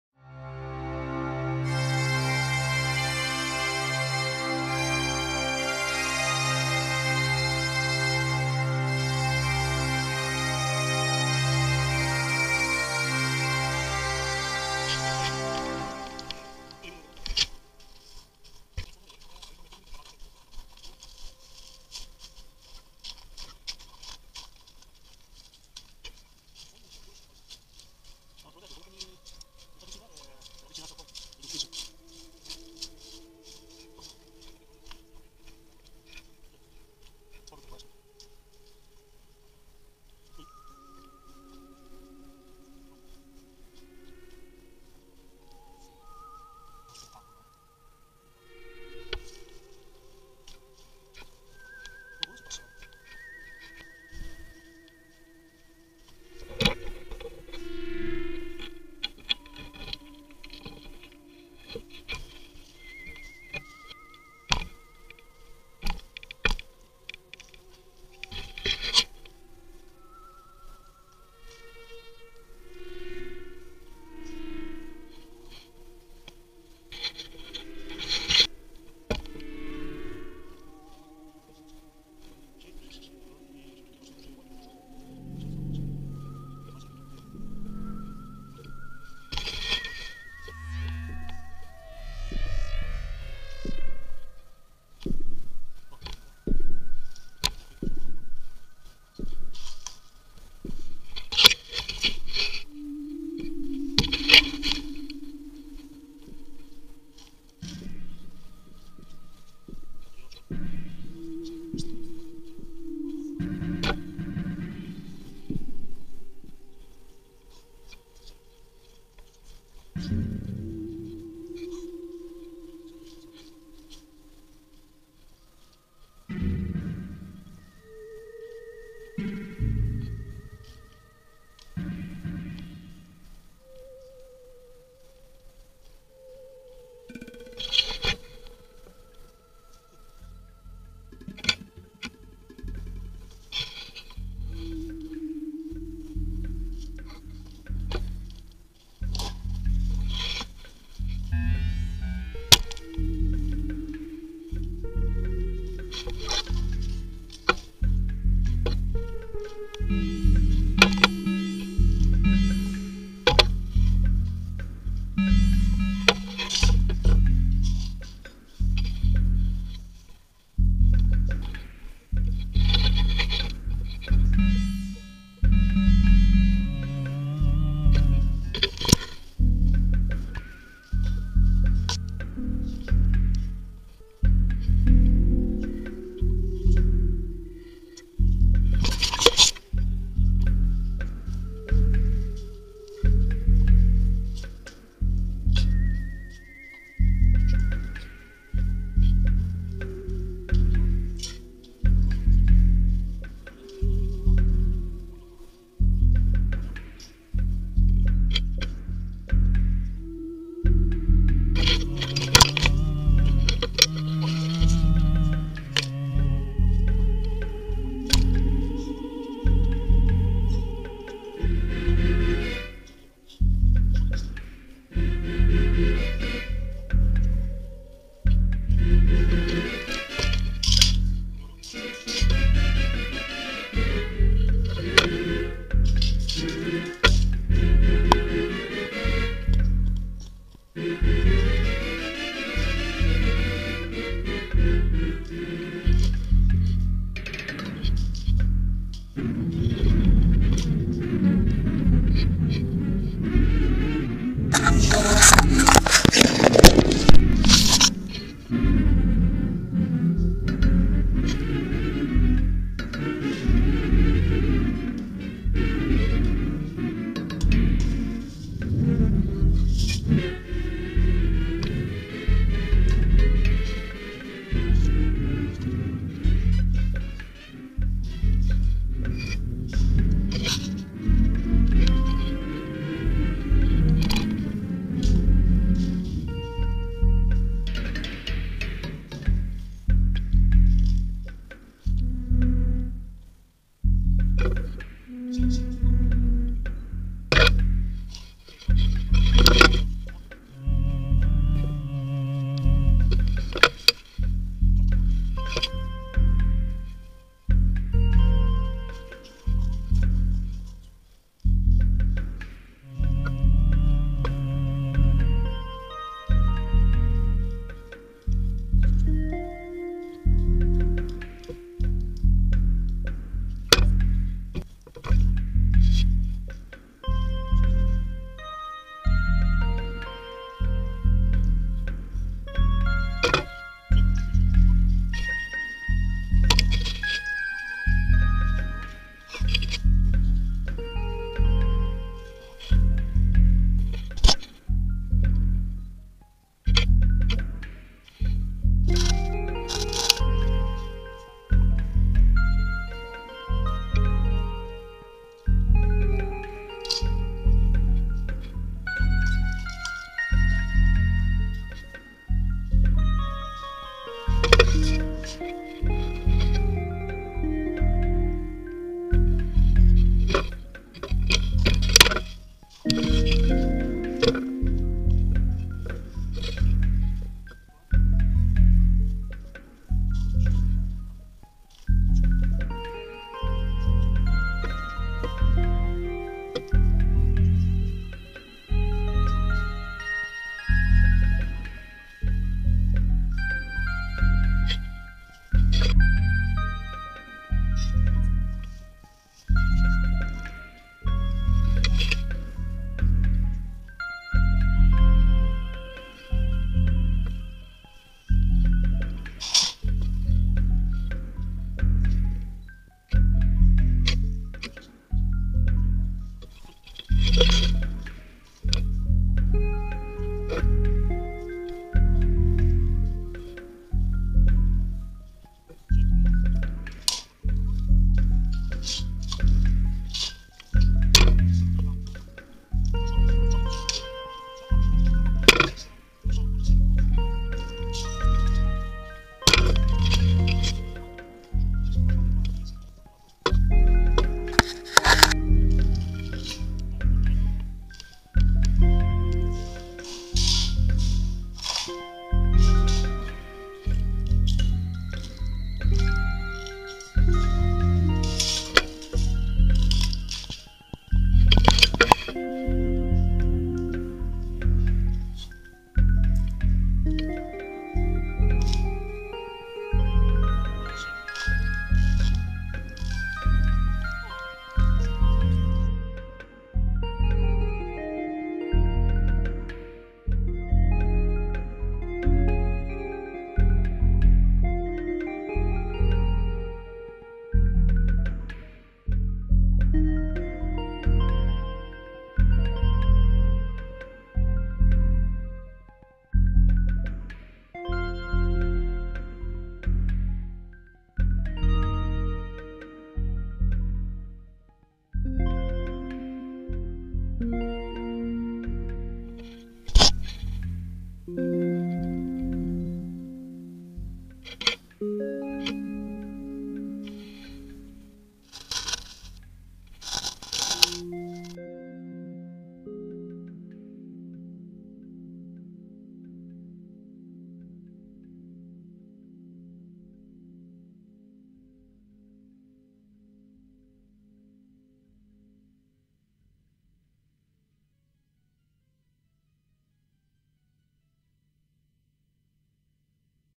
La Necchi (Pavia) FABBRICA ABBANDONATA
Come direbbe l'amico Matteo Montesi: "UNA FABBRICA TITANICA"!!! è il caso della Necchi, la storica fabbrica sita a Pavia, abbandonata al tempo.